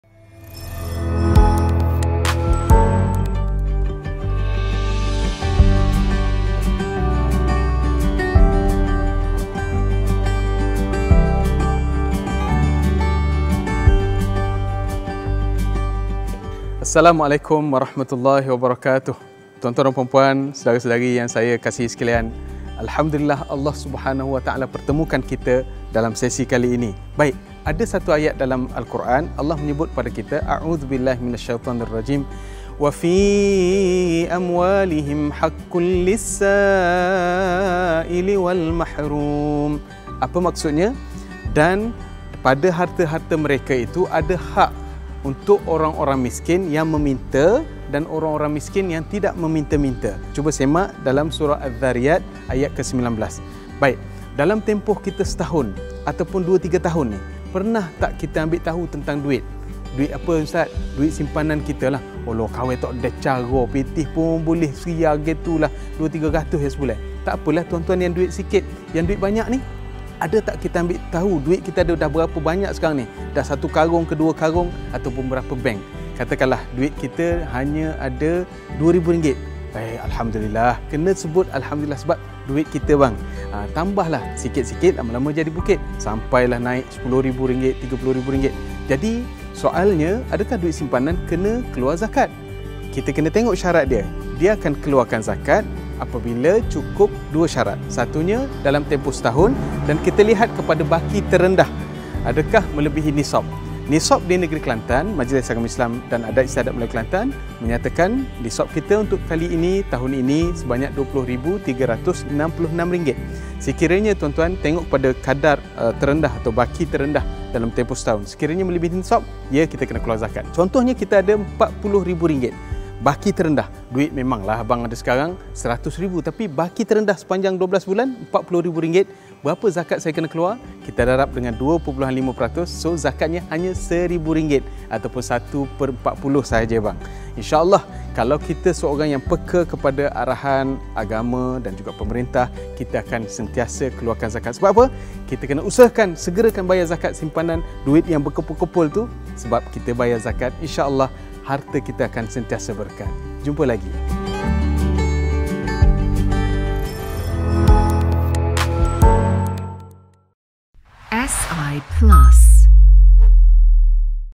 Simpanan tak banyak, kena bayar zakat juga ke? | Soal Jawab Agama
Simpanan tak banyak, kena bayar zakat juga ke?

Ikuti penjelasan daripada Ustaz Ameer Azeezy Tuan Abdullah, Ketua Pegawai Zakat MAIK dalam sesi Soal Jawab Agama kali ini.